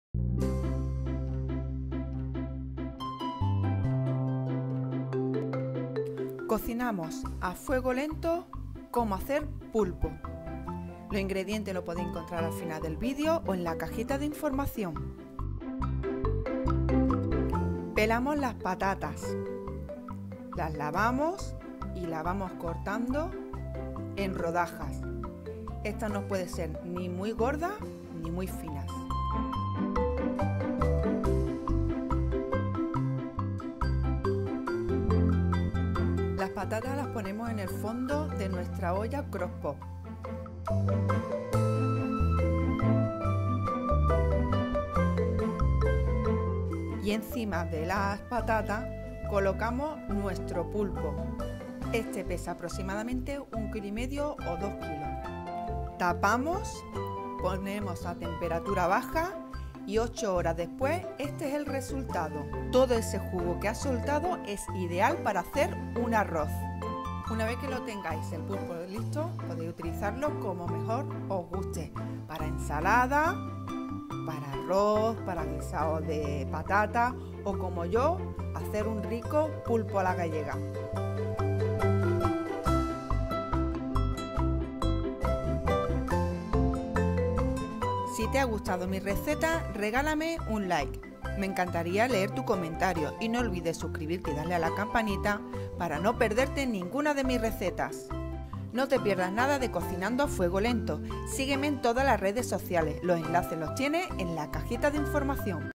COMO COCER PULPO / HOW TO COOK OCTOPUS / CROCKPOT
Como cocer pulpo y que te quede un pulpo de lo mas rico, así es como sale con la olla Crock pot.

INGREDIENTES:
Pulpo de 1,5k. o 2k.
Patatas.

INGREDIENTS:
Octopus of 1,5k. or 2k.
Potatoes.